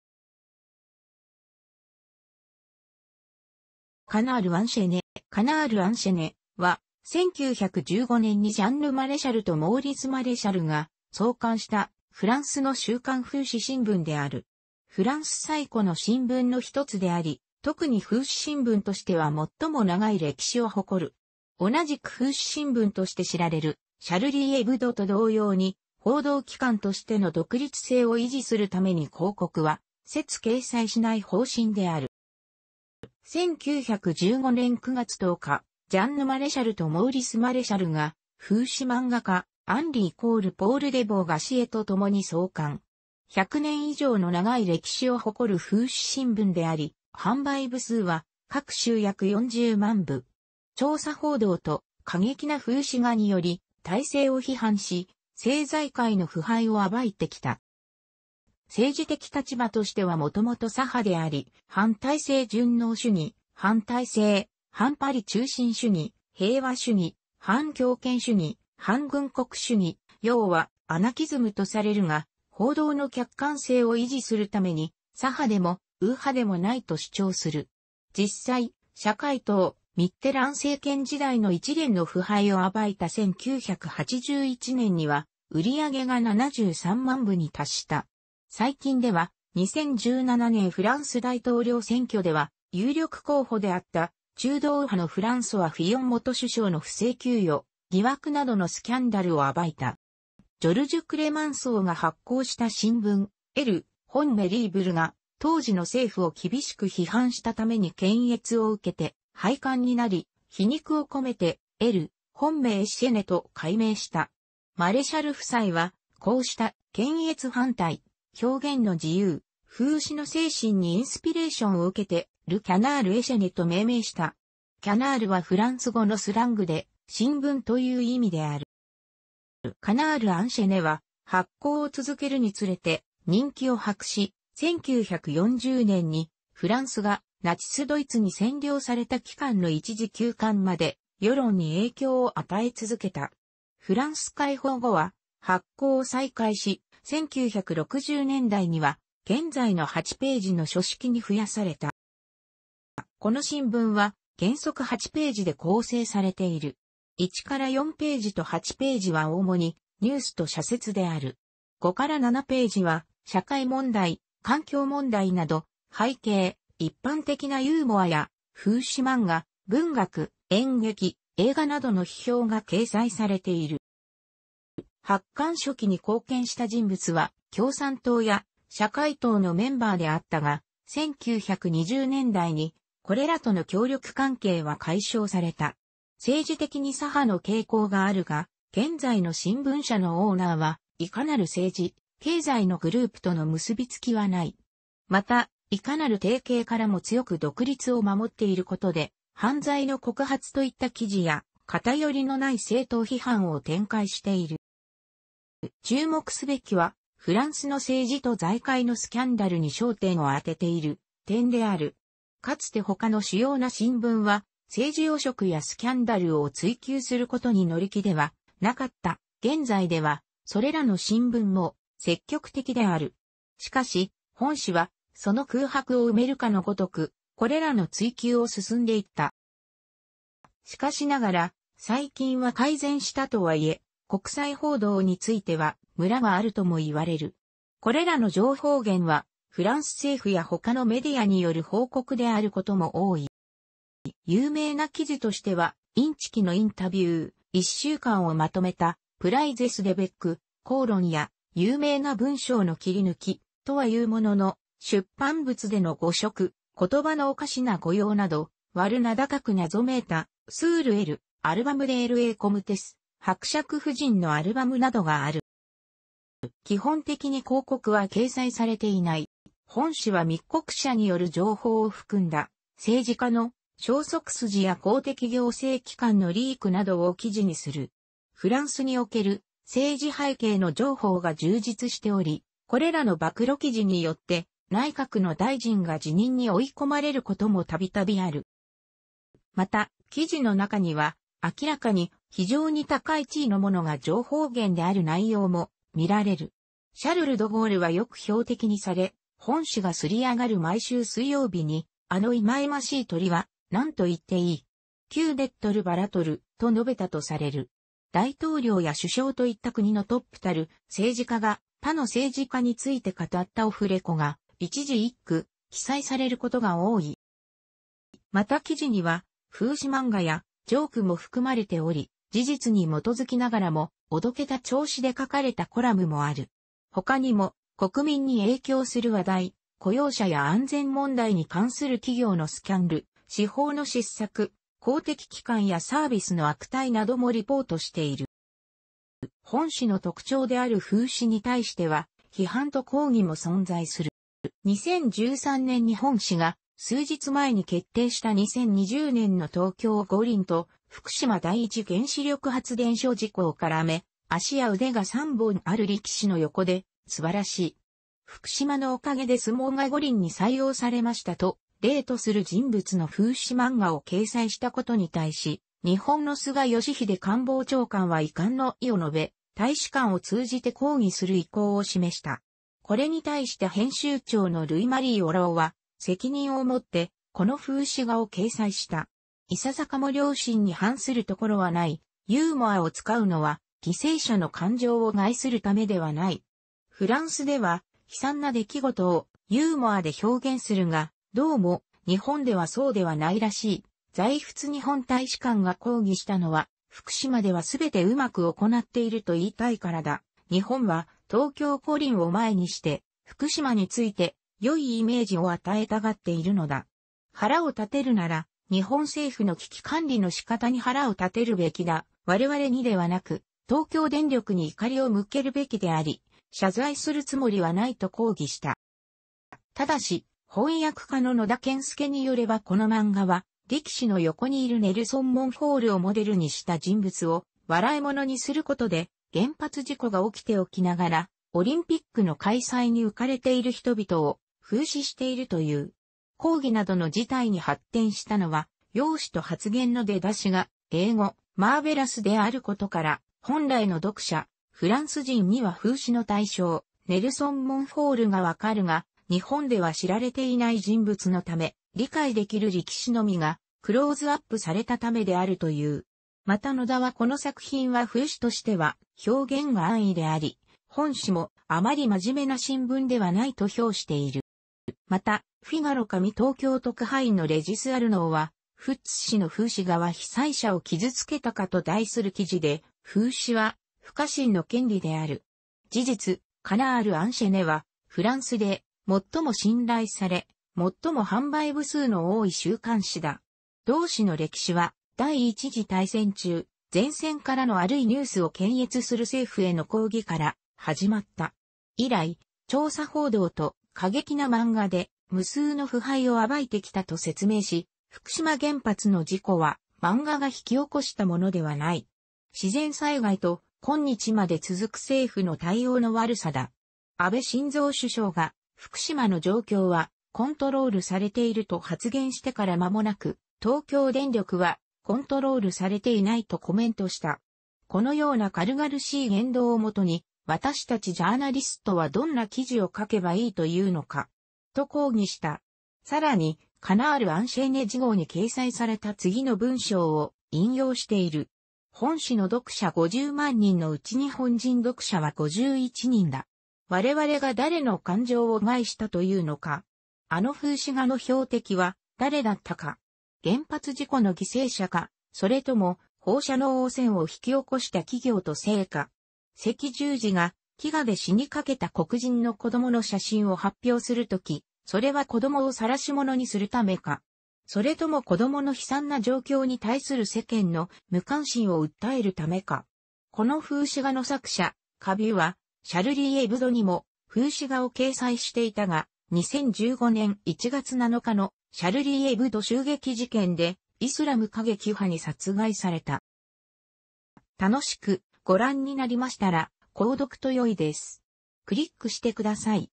カナール・アンシェネ
カナール・アンシェネ（）は、1915年にジャンヌ・マレシャルとモーリス・マレシャルが創刊したフランスの週刊風刺新聞である。フランス最古の新聞の一つであり、特に風刺新聞としては最も長い歴史を誇る。同じく風刺新聞として知られる『シャルリー・エブド』と同様に、報道機関としての独立性を維持するために広告は一切掲載しない方針である。

1915年9月10日、ジャンヌ・マレシャルとモーリス・マレシャルが風刺漫画家アンリ＝ポール・デヴォー・ガシエとともに創刊。100年以上の長い歴史を誇る風刺新聞であり、販売部数は各週約40万部。調査報道と過激な風刺画により体制を批判し、政財界の腐敗を暴いてきた。

政治的立場としてはもともと左派であり、「反体制順応主義（反体制）、反パリ中心主義、平和主義、反教権主義、反軍国主義、要はアナキズム」とされるが、報道の客観性を維持するために左派でも右派でもないと主張する。 実際、社会党ミッテラン政権時代の一連の腐敗を暴いた1981年には売上が73万部に達した。最近では、2017年フランス大統領選挙では有力候補であった中道右派のフランソワ・フィヨン元首相の不正給与疑惑などのスキャンダルを暴いた。

ジョルジュ・クレマンソーが発行した新聞『"L'homme libre" (自由の人)』が当時の政府を厳しく批判したために検閲を受けて廃刊になり、皮肉を込めて『"L'homme enchaîné" (鎖につながれた人)』と改名した。マレシャル夫妻はこうした検閲反対、表現の自由、風刺の精神にインスピレーションを受けて、『"Le Canard enchaîné" (鎖につながれたカモ)』と命名した。"Canard (カモ)" はフランス語のスラングで「新聞」という意味である。

『カナール・アンシェネ』は発行を続けるにつれて人気を博し、1940年に、フランスがナチス・ドイツに占領された期間の一時休刊まで、世論に影響を与え続けた。フランス解放後は発行を再開し、1960年代には、現在の8ページの書式に増やされた。

この新聞は、原則8ページで構成されている。1～4ページと8ページは主にニュースと社説である。5～7ページは社会問題（環境問題など）、背景、一般的なユーモアや風刺漫画、文学・演劇・映画などの批評が掲載されている。

発刊初期に貢献した人物は、共産党や社会党のメンバーであったが、1920年代にこれらとの協力関係は解消された。政治的に左派の傾向があるが、現在の新聞社のオーナーは、いかなる政治・経済のグループとの結びつきはない。また、いかなる提携からも強く独立を守っていることで、犯罪の告発といった記事や、偏りのない政党批判を展開している。

注目すべきは、フランスの政治と財界のスキャンダルに焦点を当てている点である。かつて他の主要な新聞は、政治汚職やスキャンダルを追及することに乗り気ではなかった（現在では、それらの新聞も積極的である）。しかし、本紙はその空白を埋めるかのごとく、これらの追及を進んで行った。

しかしながら、最近は改善したとはいえ、国際報道についてはムラがあるともいわれる。これらの情報源は、フランス政府や他のメディアによる報告であることも多い。

有名な記事としては、インチキのインタビュー、一週間をまとめた「Prises de Bec（口論）」や、有名な文章の切り抜き（とはいうものの、出版物での誤植・言葉のおかしな誤用など）、悪名高く謎めいた「Sur l'Album de la Comtesse（伯爵夫人のアルバム）」などがある。

基本的に広告は掲載されていない。

本紙は密告者による情報を含んだ、政治家の「消息筋」や公的行政機関のリークなどを記事にする。フランスにおける政治背景の情報が充実しており、これらの暴露記事によって、内閣の大臣が辞任に追い込まれることもたびたびある。

また、記事の中には、明らかに、非常に高い地位の者が情報源である内容も見られる。シャルル・ド・ゴールはよく標的にされ、本紙が刷り上がる毎週水曜日に「あの忌々しい鳥は何と言っている?（que dit le volatile?）」と述べたとされる。大統領や首相といった国のトップたる政治家が、他の政治家について語ったオフレコが、一字一句記載されることが多い。

また記事には、風刺漫画やジョークも含まれており、事実に基づきながらも、おどけた調子...